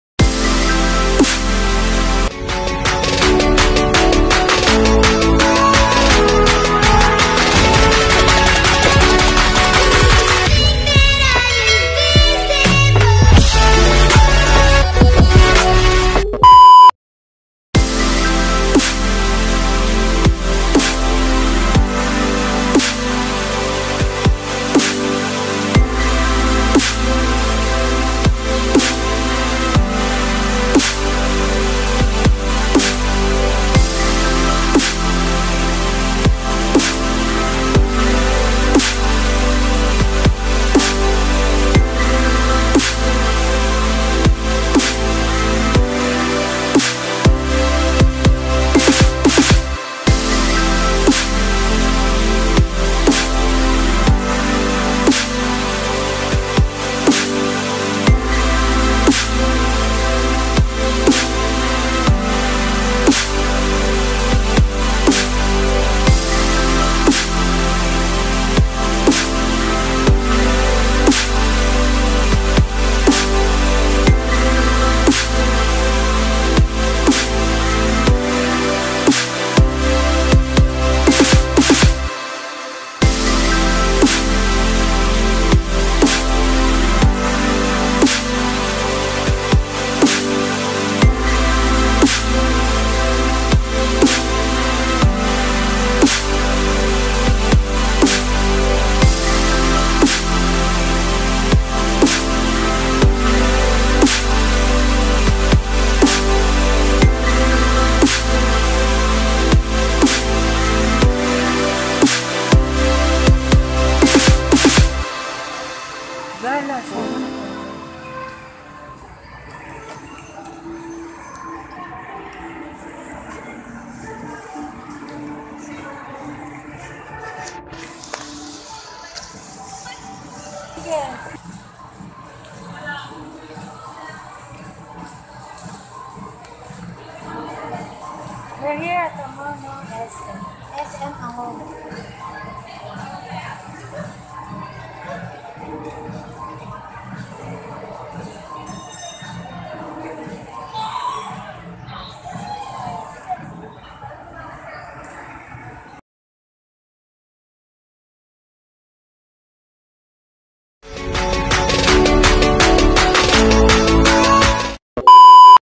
Pasyal sa SM
For content purposes only, we went to SM just to claim her ATM card and while waiting for withdrawal of her salary,,we just walk around inside the Mall.Thank you for watching, and GOD BLESS!